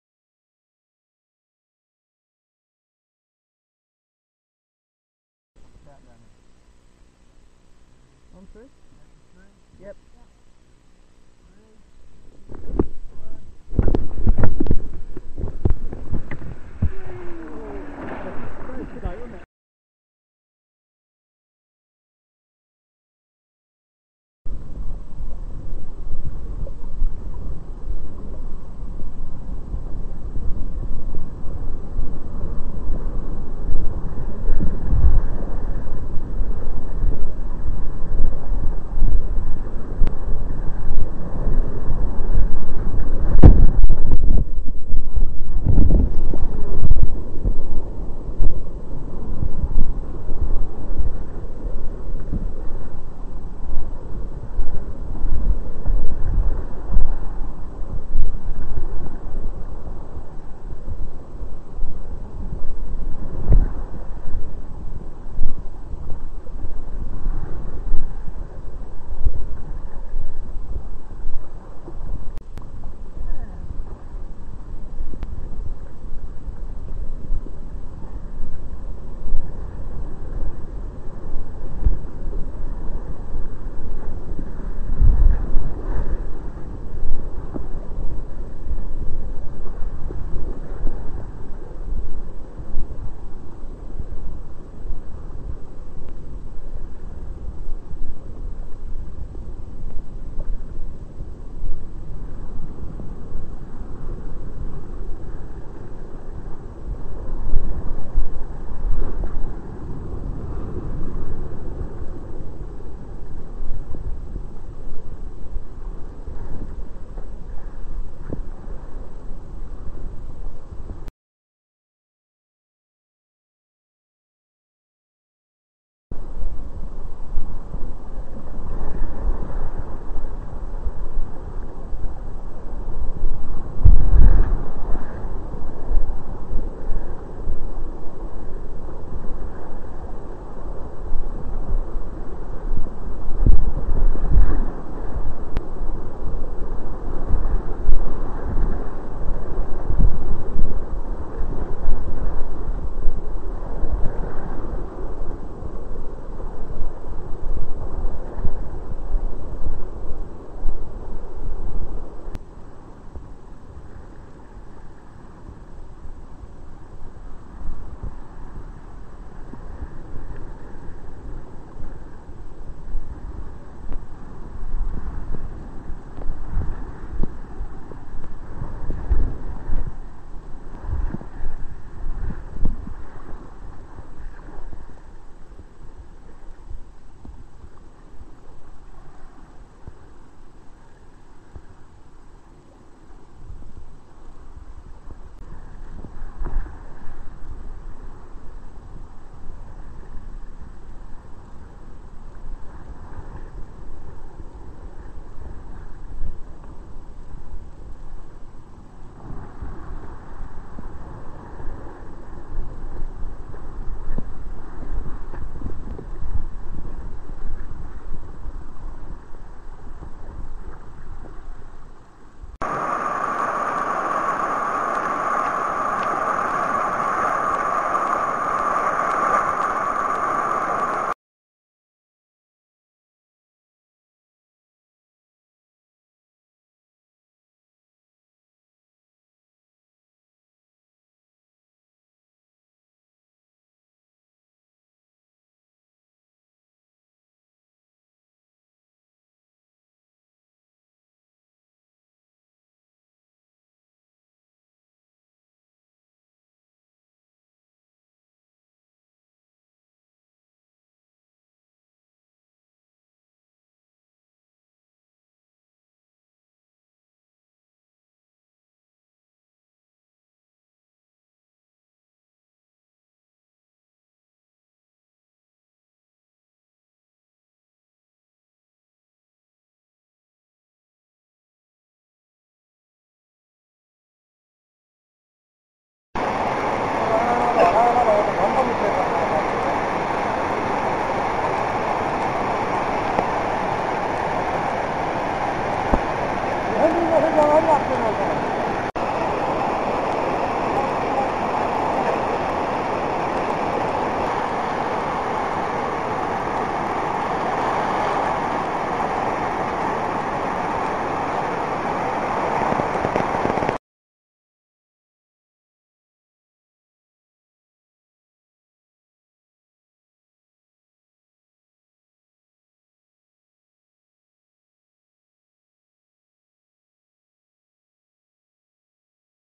Sun Kosi River - Sukute Beach to Chatra - Part 2
Footage from a trip on the Sun Kosi River between Sukute Beach and Chatra in November 2010